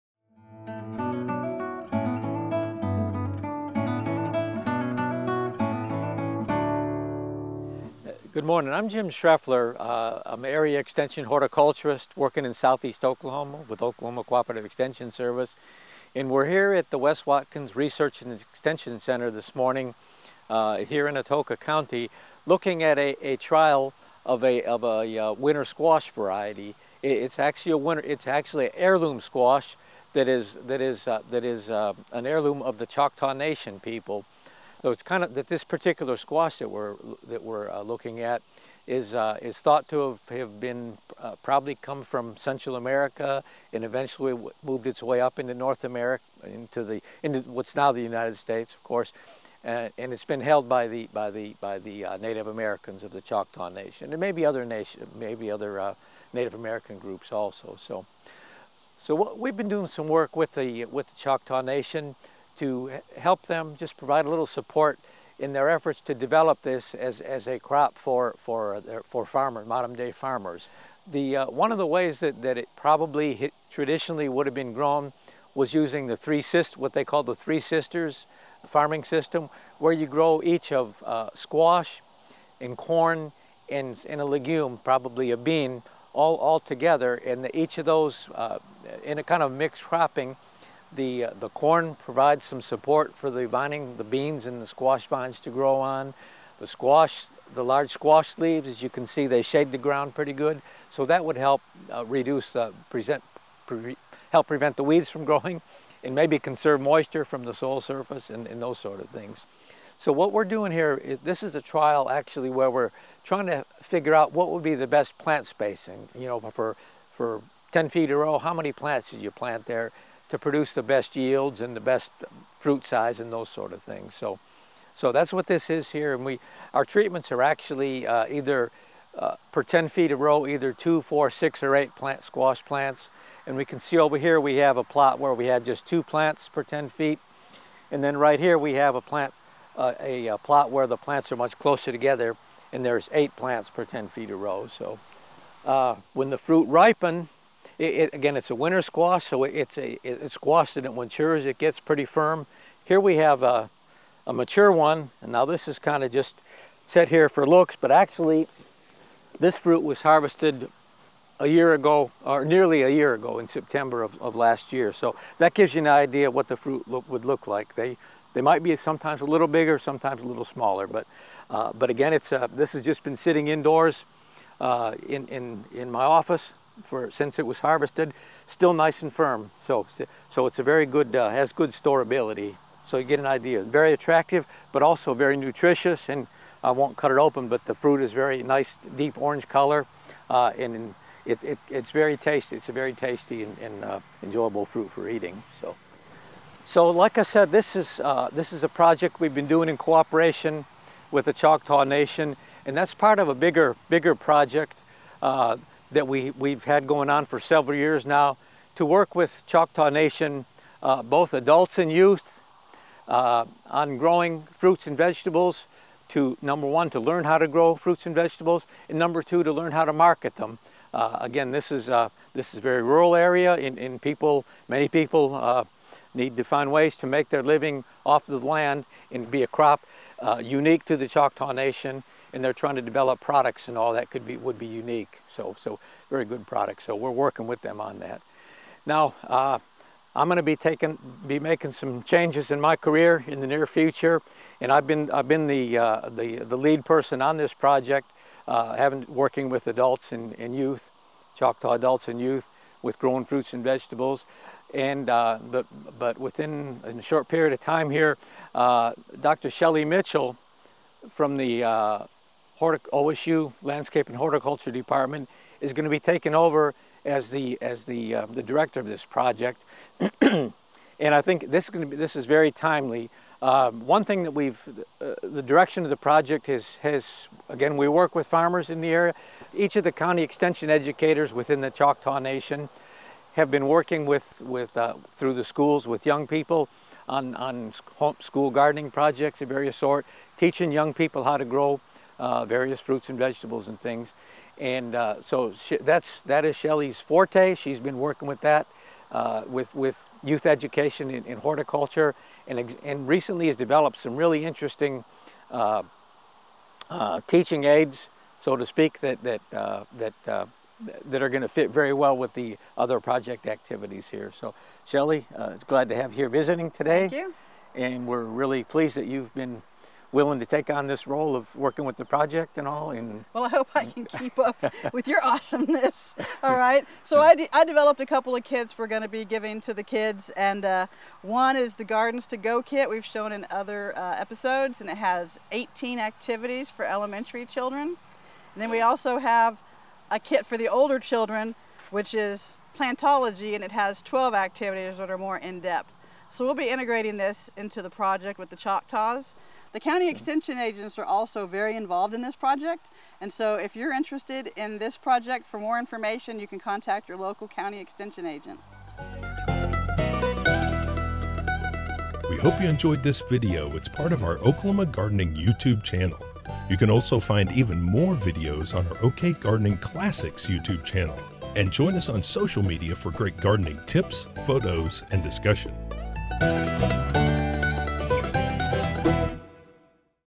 Heirloom Choctaw Squash Research for the Choctaw Tribe
Jim Shrefler, S.E. Area Horticulture Specialist at OSU, updates us on the research plots of the native heirloom squash to the Choctaw Tribe and announces his retirement from OSU. Jim introduces Shelley Mitchell, Associate Extension Specialist, as the new leader for this research project.

Questions?